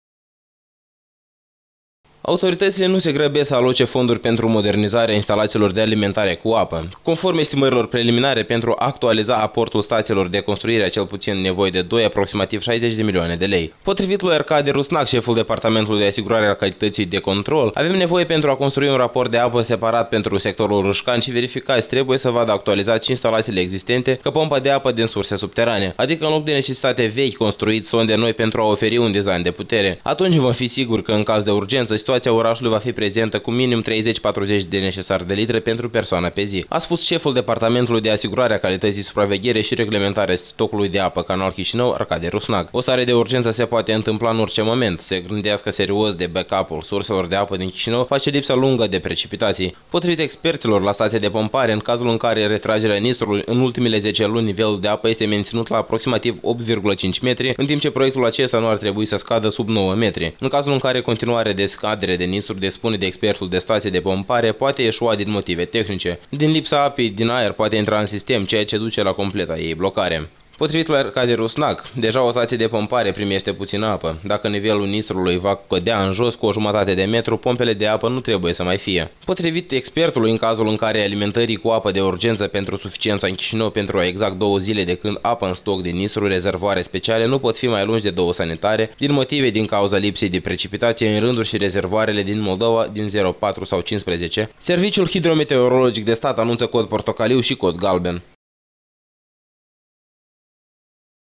Nistrul îsi pierde apa
Autoritățile nu se grăbesc să aloce fonduri pentru modernizarea instalațiilor de alimentare cu apă. Conform estimărilor preliminare pentru a actualiza aportul stații și construirea de cel puțin o nevoie de noi aproximativ 60 milioane lei.